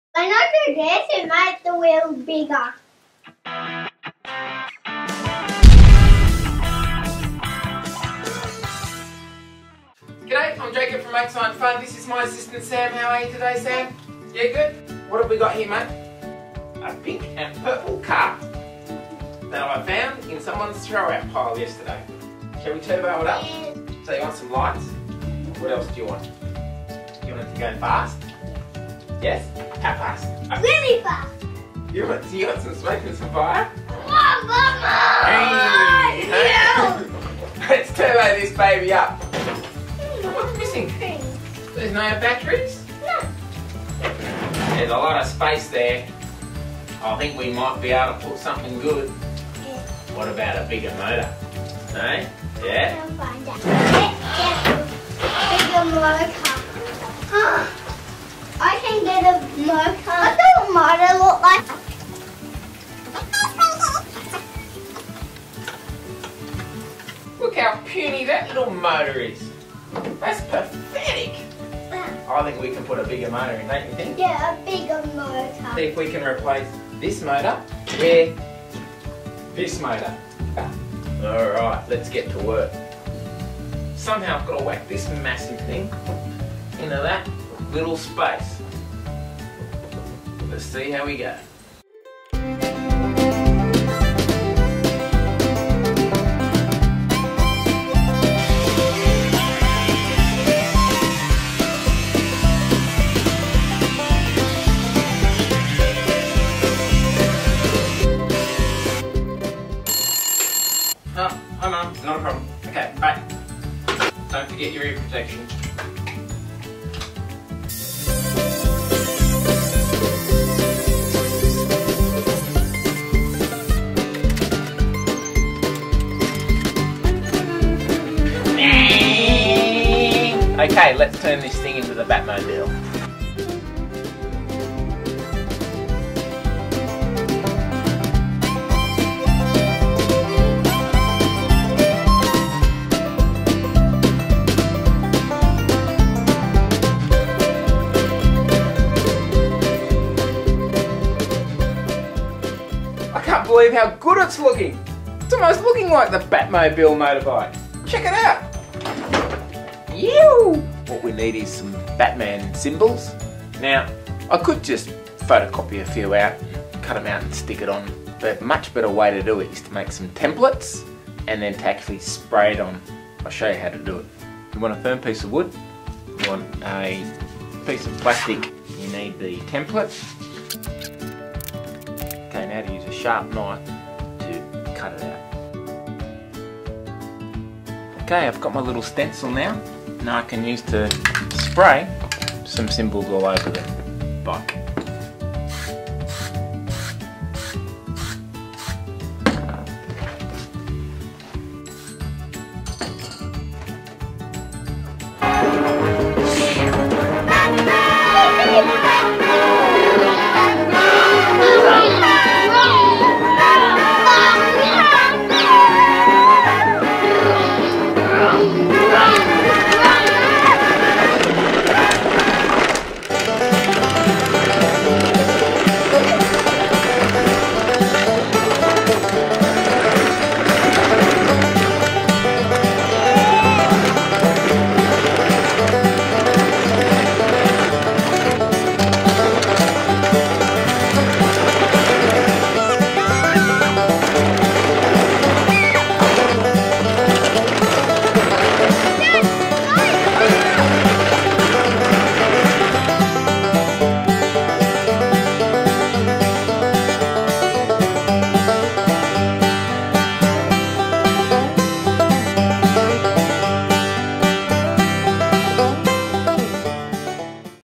How to make a batmobile - Make Science Fun
Turbo Up My Toy! Batmans Batmobile. Jacob and Sam turbo up a toddlers pink and purple toy car and transform it into a much more powerful machine worthy of Batman!